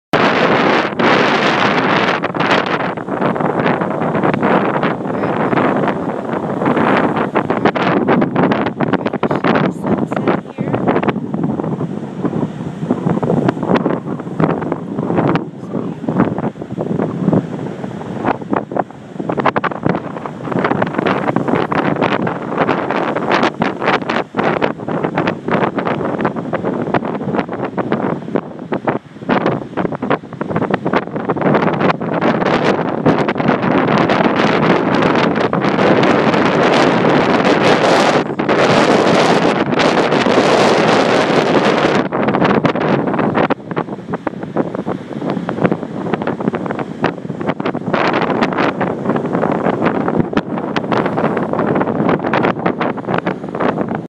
Garrapata State Park, Big Sur, California: Sunset Photography
Photographer Marcus Reinkensmeyer with tripod, attempting to capture ward sunset light at rugged Garrapata State Park. Another great day in Big Sur, California,